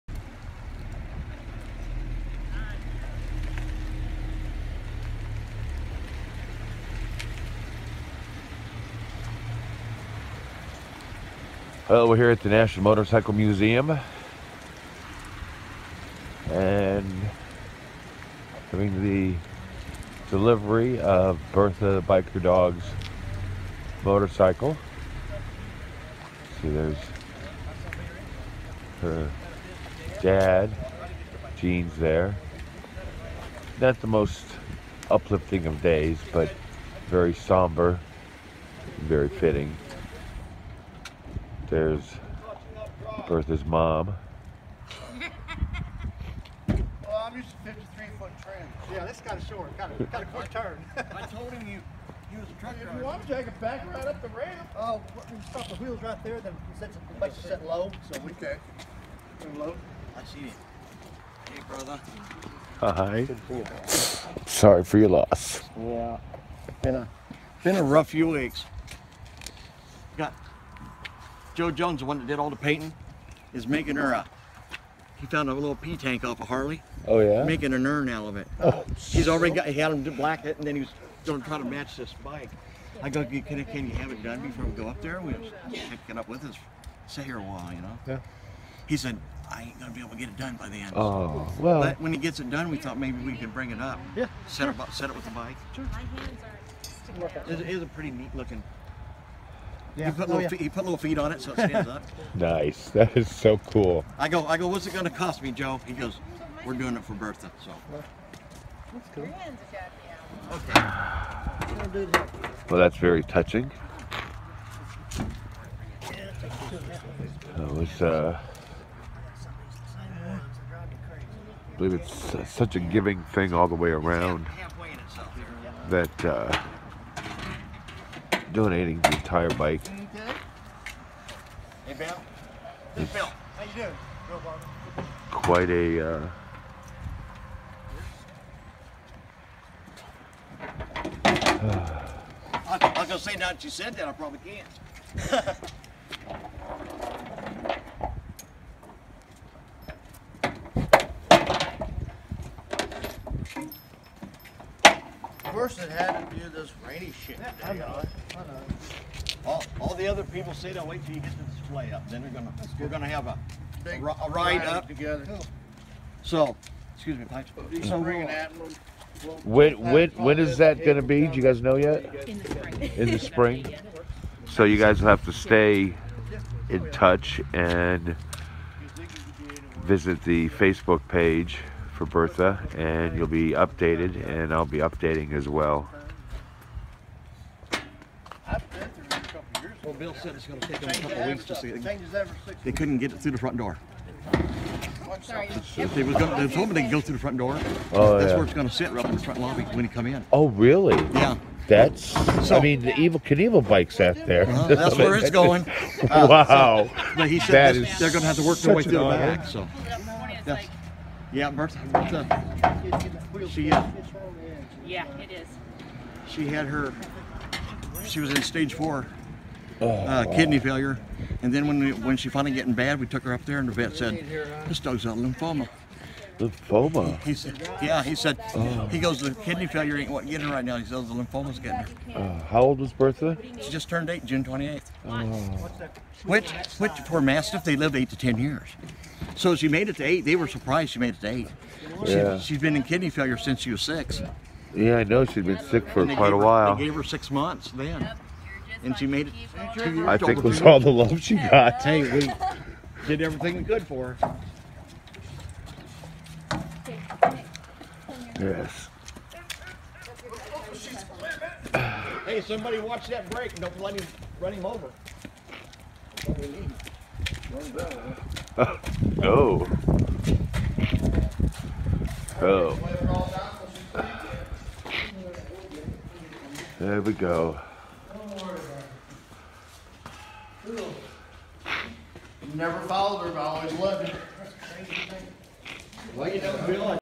Bertha the Biker Dog’s Motorcycle arrives
Her motorcycle being donated to the National Motorcycle Museum in Anamosa, Iowa